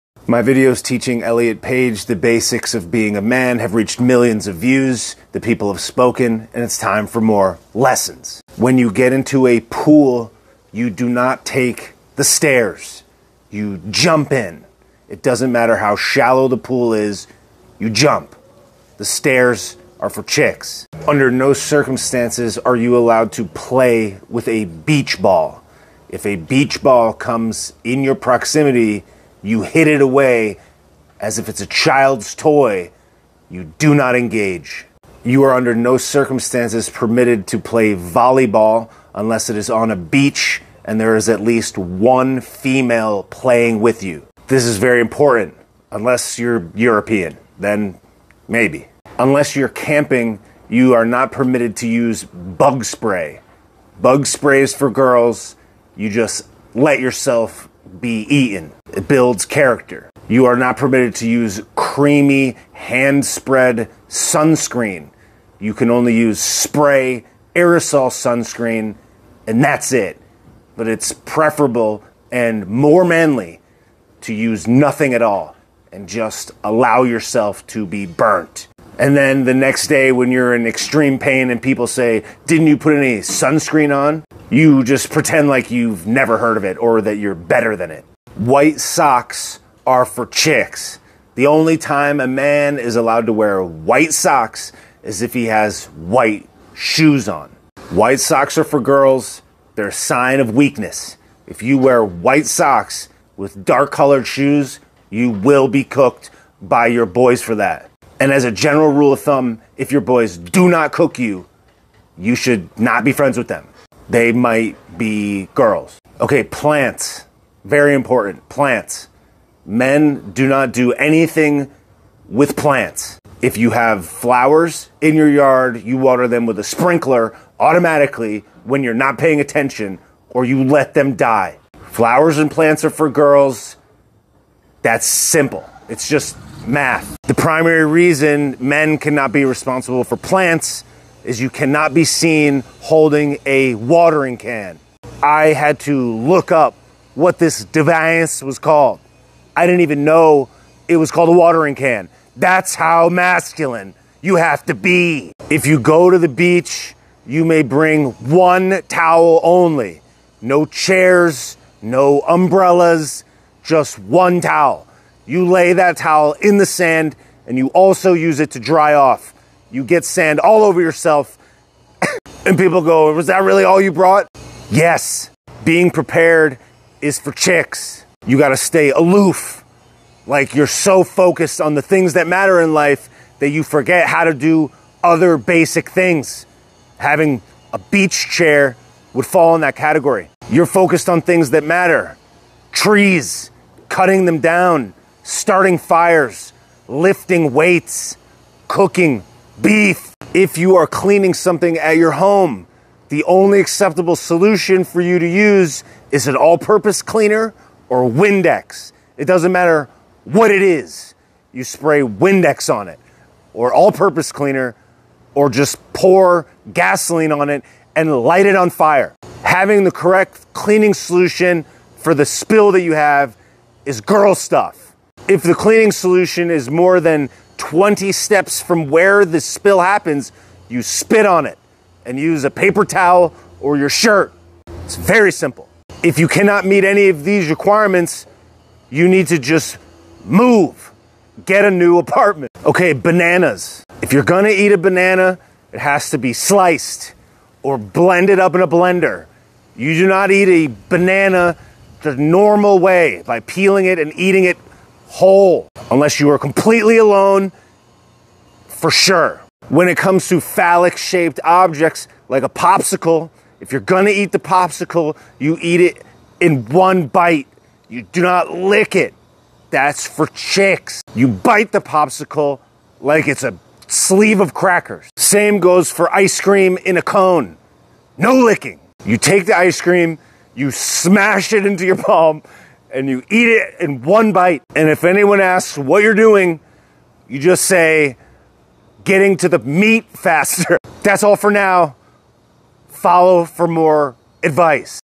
How to Be a Man
Continuing my series on teaching men how to man, this video covers how to enter a pool, beach and summer activities, acceptable sock colors, how to clean, why plants are for chicks only and how to eat a banana.